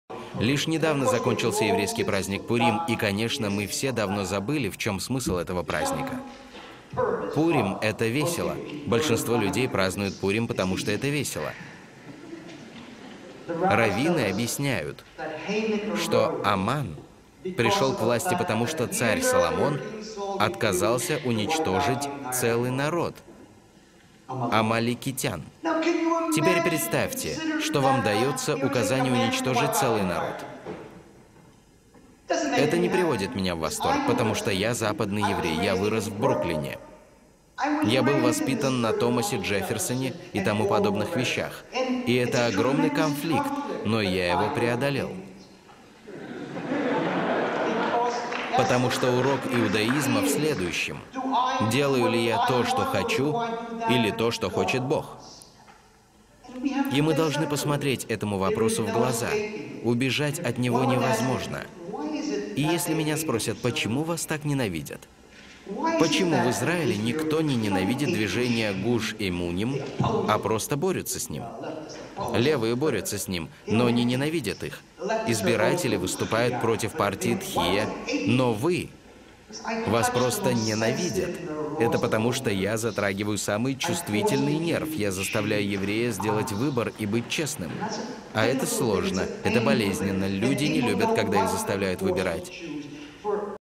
8.11 Рабби Меир Кахане הי"ד. Урок иудаизма в следующем: делаю ли я то, что захочу...
Рабби Меир Кахане. Урок иудаизма в следующем: делаю ли я то, что захочу, или то, чего хочет Б-г.